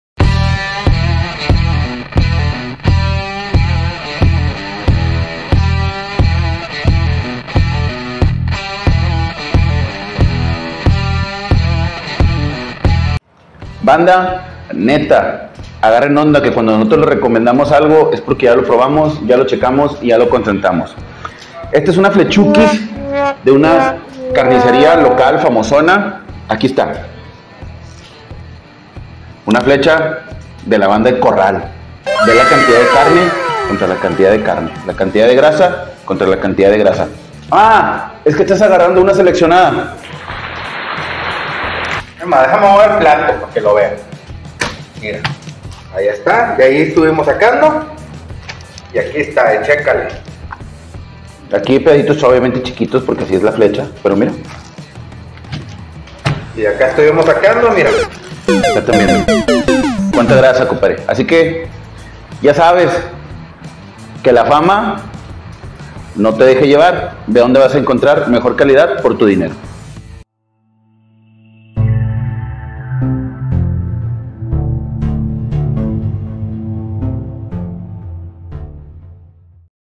Oso Pirata "El Reto Corral"
Comparamos la flecha de "Súper Carnes Corral", con la flecha de una carnicería de cadena muy famosa en la ciudad.

vean el reto, y no compren nombre, compren calidad.